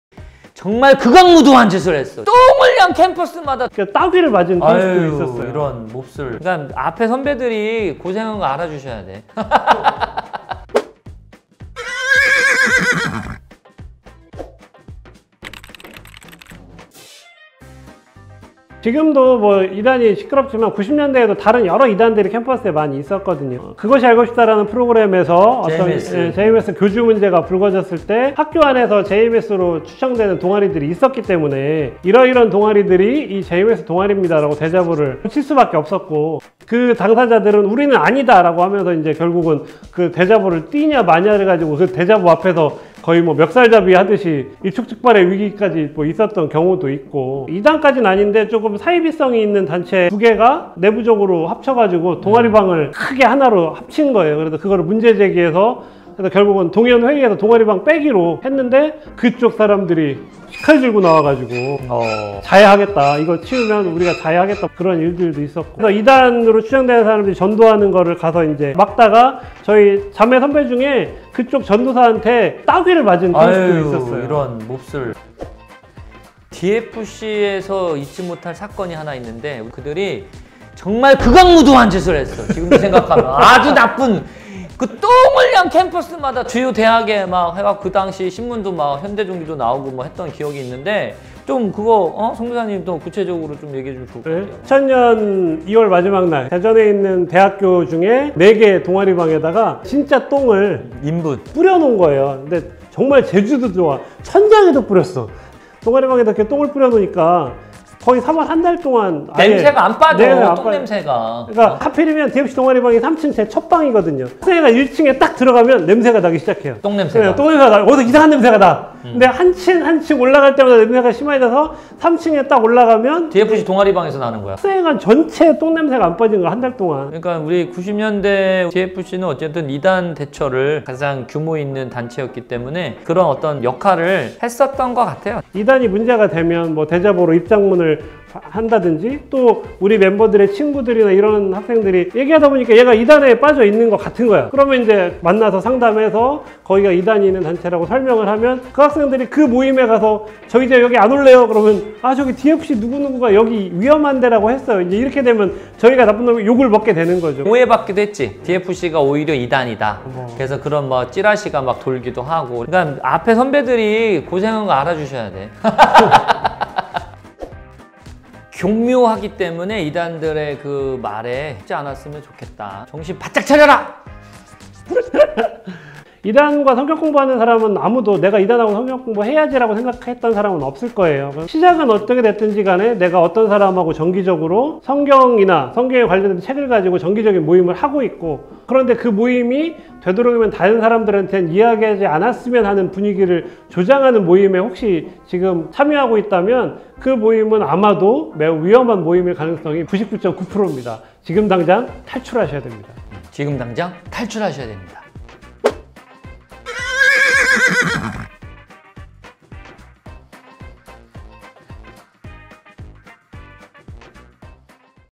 [D학생활외전] 그 때 이단들은 진짜 어마무시 했어😨그 유명한 x테러 사건의 전말ㅣ 라떼는말이야 3편
D학생활 외전ㅣ
90년대 학번 라떼들이 말하는 그때 그 시절 이야기!
그 시절 사람들의 공감파티~!!!
애들아 말이야 라떼는 말이야 으이~?!🤣🤣ㅎㅎㅎㅎㅎㅎㅎㅎㅎㅎㅎㅎㅎㅎㅎ

# DFC에 대해서 더 알고 싶다면?